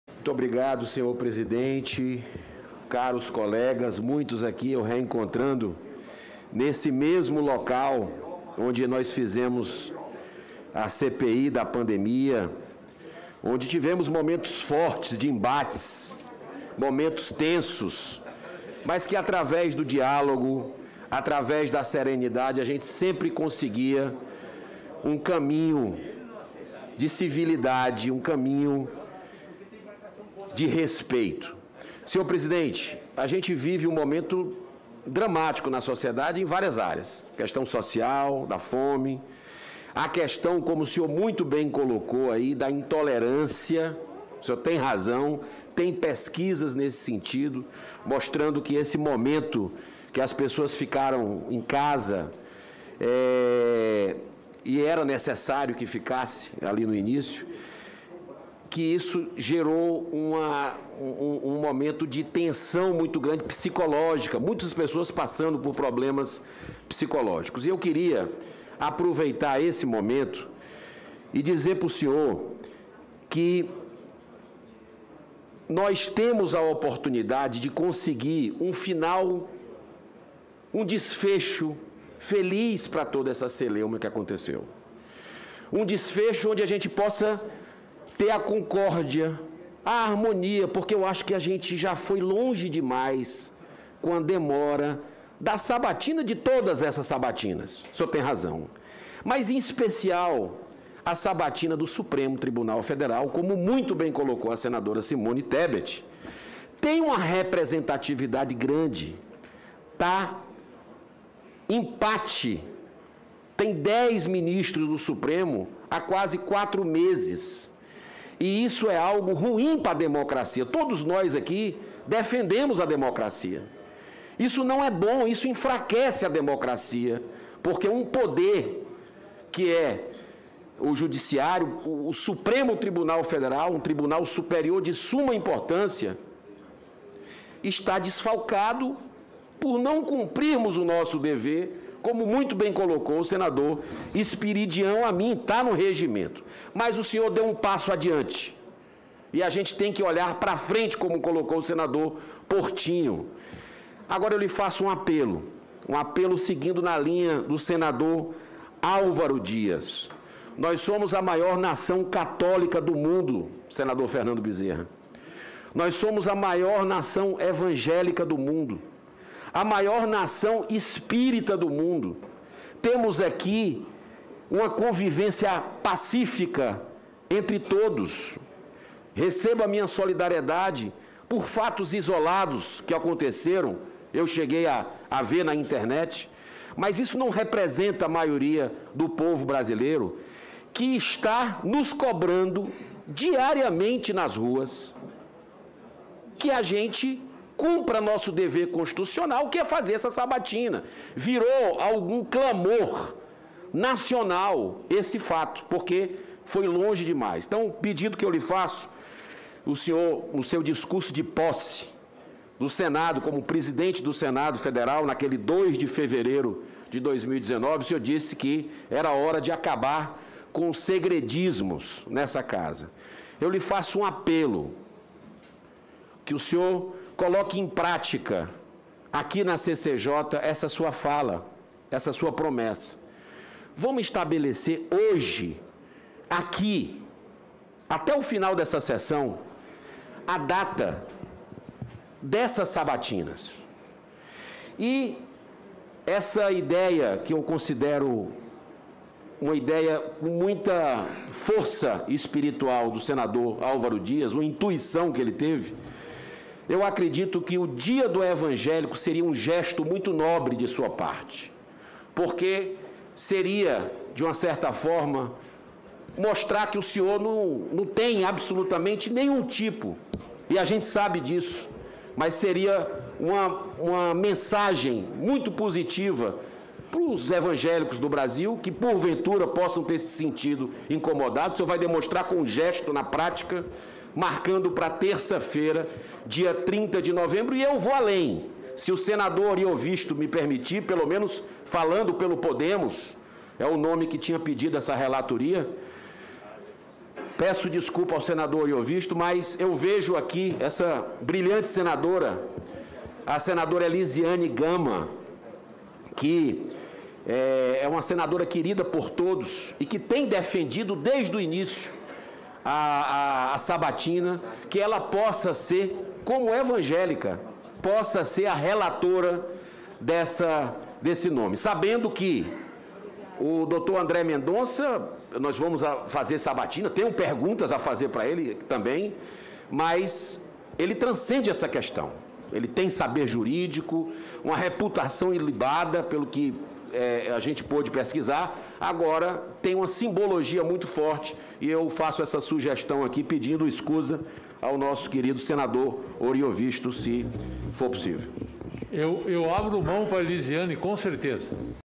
"Supremo está desfalcado por não cumprirmos regimento", crítica Girão
O senador Eduardo Girão (Podemos) pediu ao presidente da Comissão de Constituição e Justiça (CCJ), senador Davi Alcolumbre (DEM), que a sabatina do ex-ministro André Mendonça  para o Supremo Tribunal Federal (STF) aconteça no Dia do Evangélico, comemorado na próxima terça-feira, 30. Girão fez côro à proposta defendida também pelo senador Álvaro Dias, seu correligionário.

Alcolumbre anunciou nesta quarta-feira (24) que as sabatinas dos dez indicados a cargos públicos, dentre eles André Mendonça, deve acontecer em um esforço concentrado entre os dias 30 de novembro e 3 de dezembro.